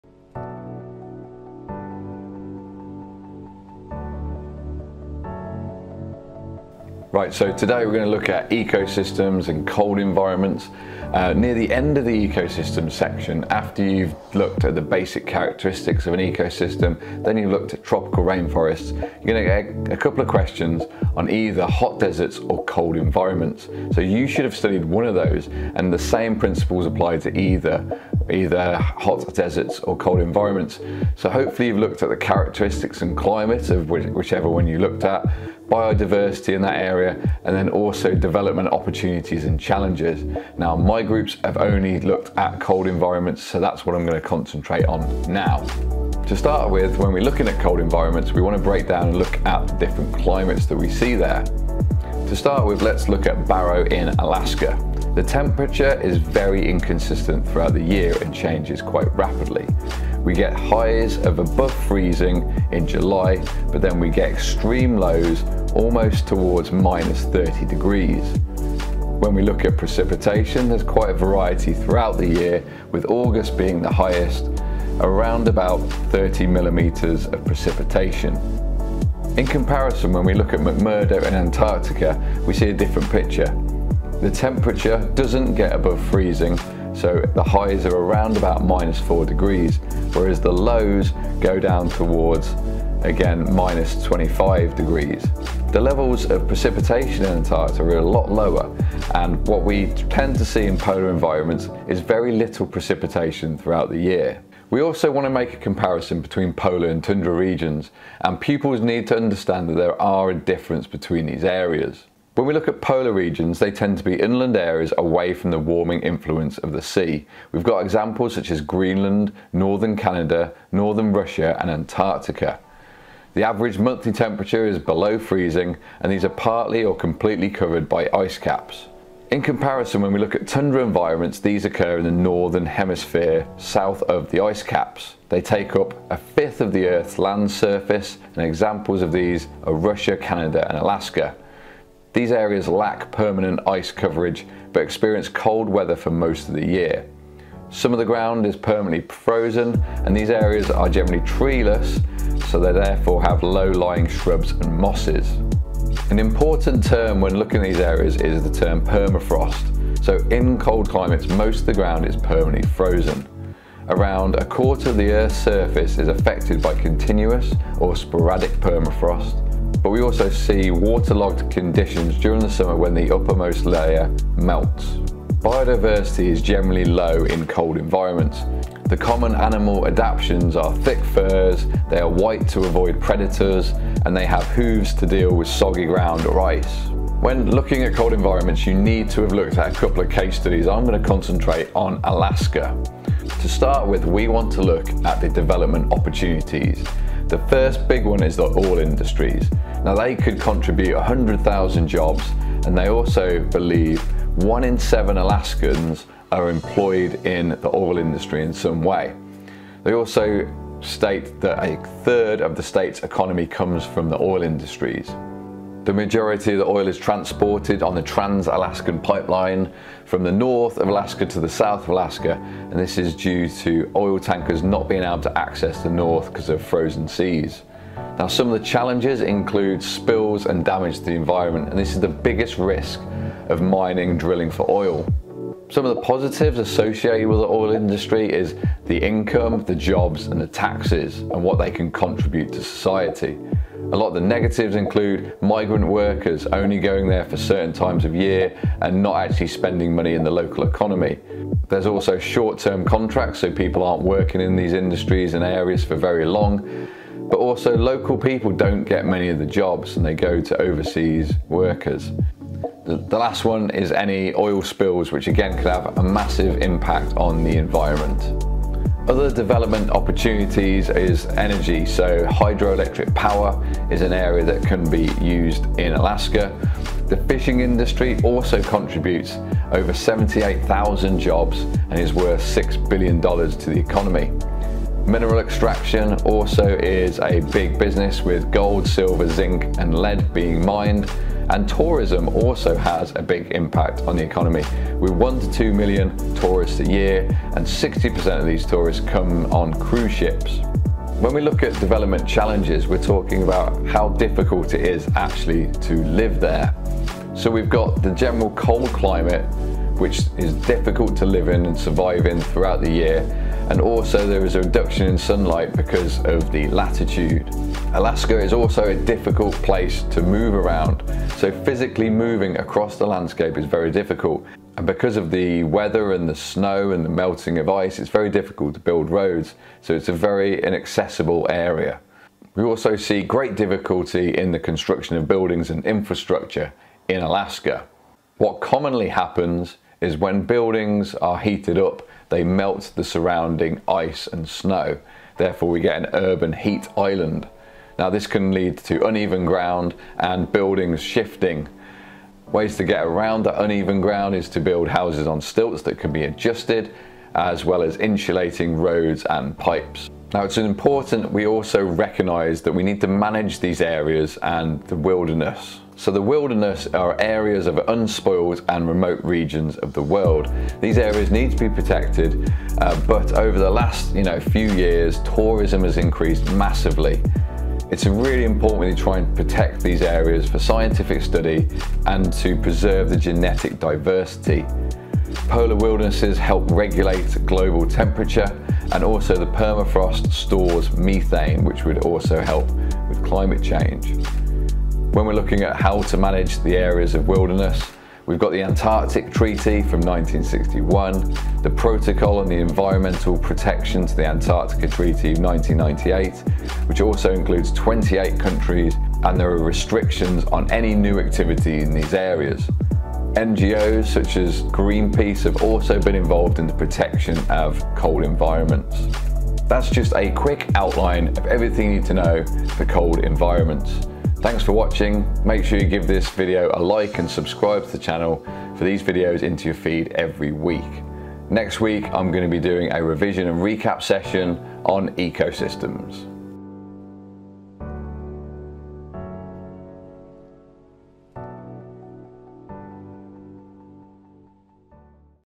Cold Environments - SUNDAY MORNING COFFEE - AQA GCSE 9-1 Geography 2021
Timestamps:
01:05 Exam Updates
01:30 Cold Environments
02:25 Climate
03:31 Polar v Tundra
04:45 Development Opportunities
06:40 Development Challenges
07:45 Wilderness Protection